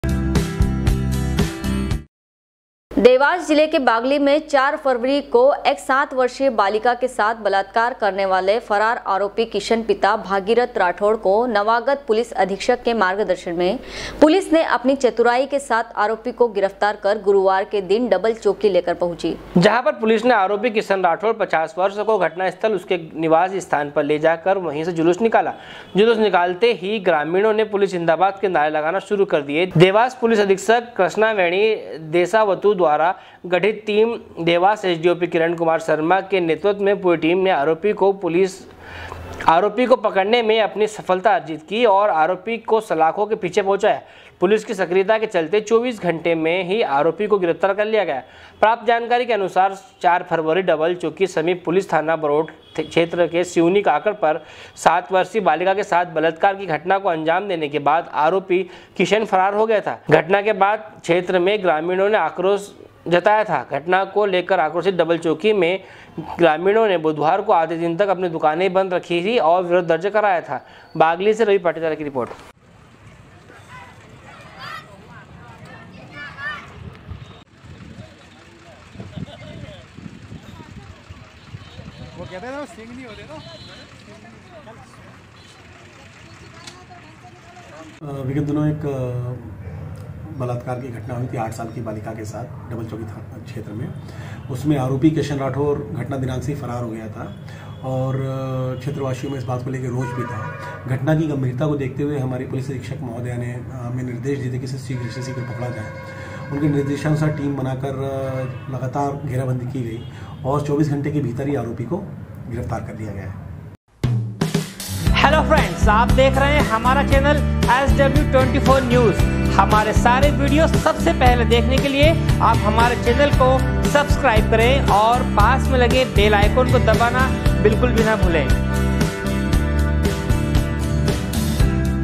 sw24news bagli, 08.02.2020
देवास जिले के बागली में 4 फरवरी को एक सात वर्षीय बालिका के साथ बलात्कार करने वाले फरार आरोपी किशन पिता भागीरथ राठौर को नवागत पुलिस अधिक्षक के मार्गदर्शन मे पुलिस ने अपनी चतुराई के साथ आरोपी को गिरफ्तार कर गुरुवार के दिन डबल चैकी लेकर पहुंची बागली से रवि पाटीदार की रिपोर्ट#sw24news